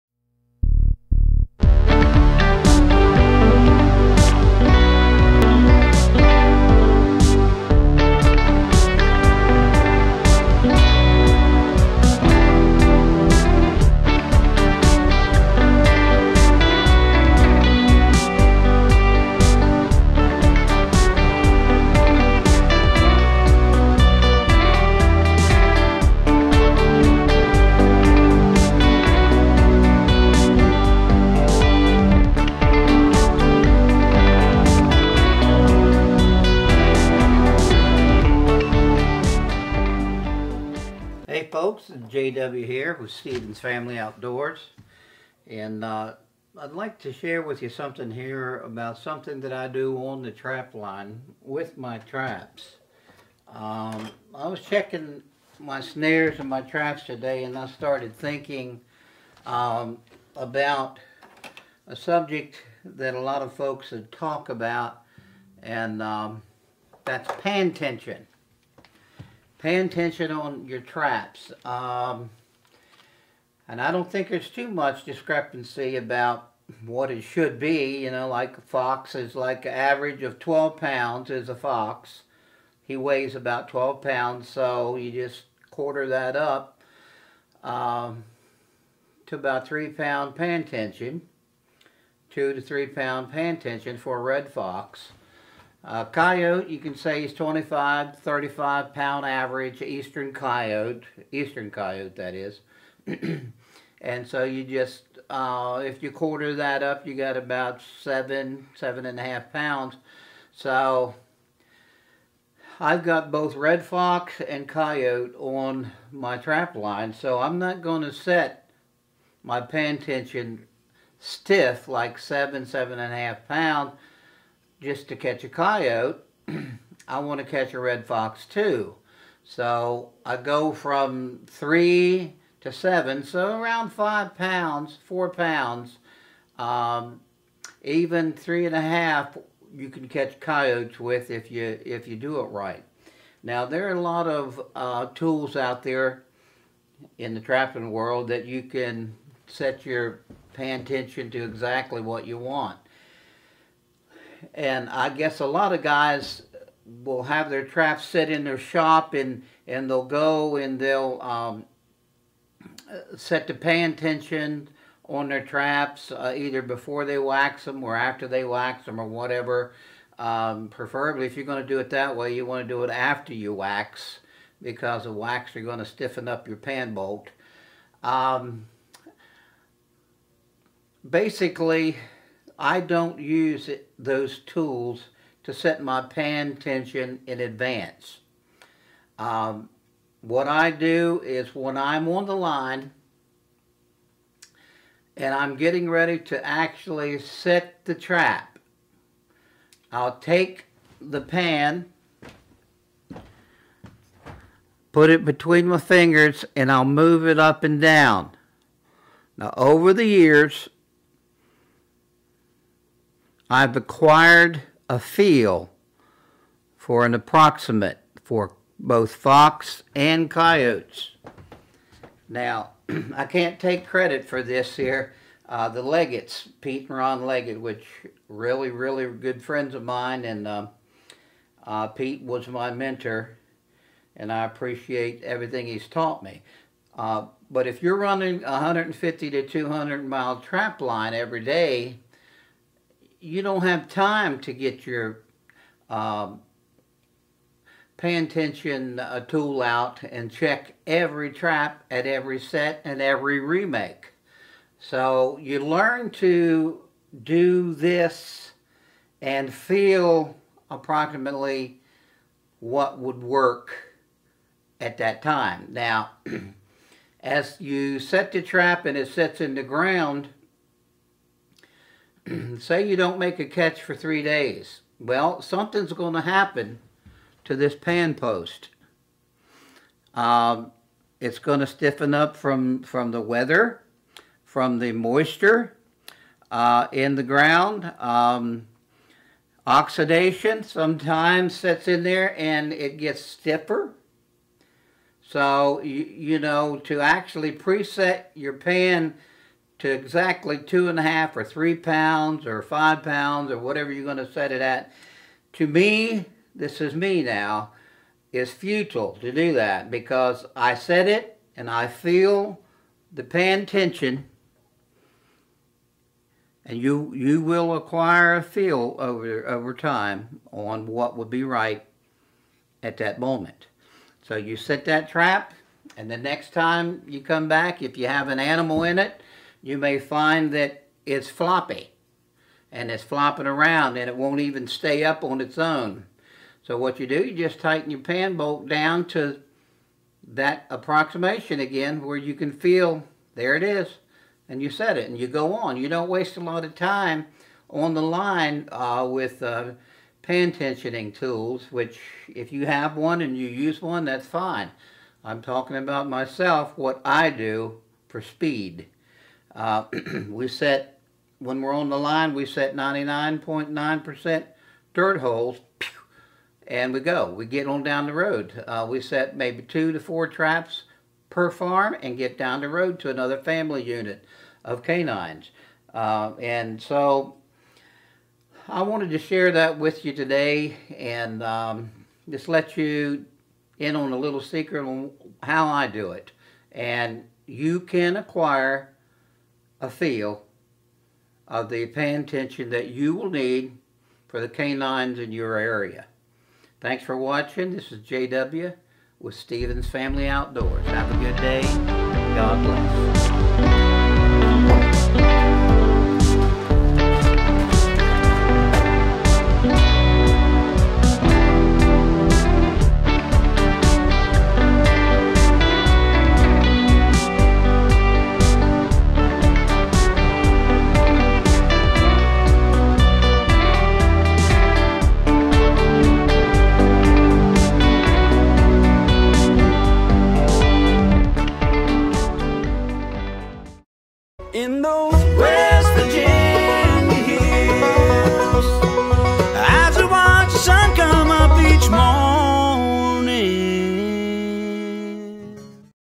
Setting Trap pan tension
JW of Stevens Family Outdoors shares a trapline tip on his popular "The Ole' Man Trappin' Series"
     Come along as he shares how he sets his trap pan tension with no special tools.